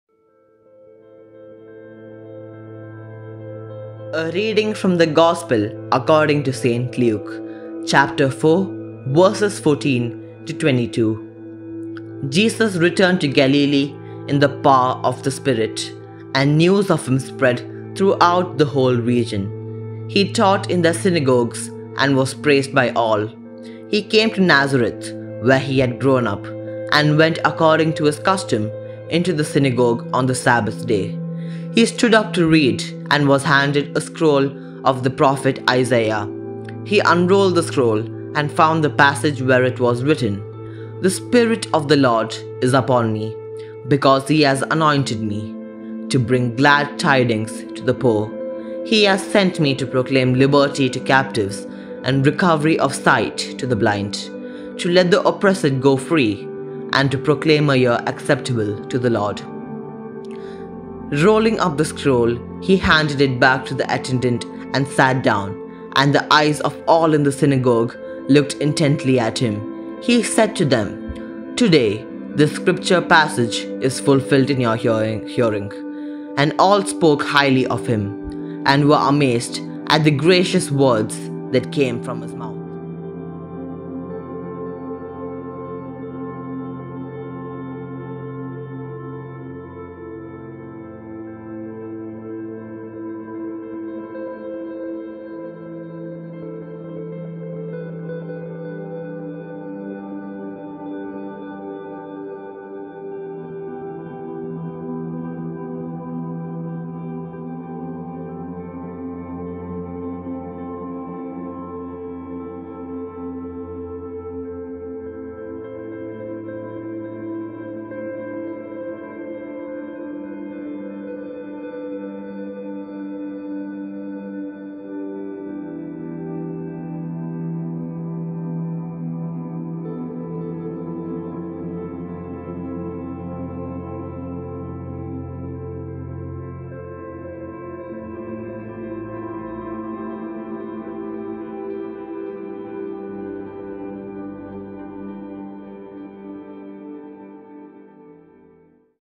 Jan 7, 2021 Luke 4:14-22 / Today's Catholic Bible Reading & Gospel Reflec. / Scripture is fulfilled
Daily Catholic Mass reading
Daily Gospel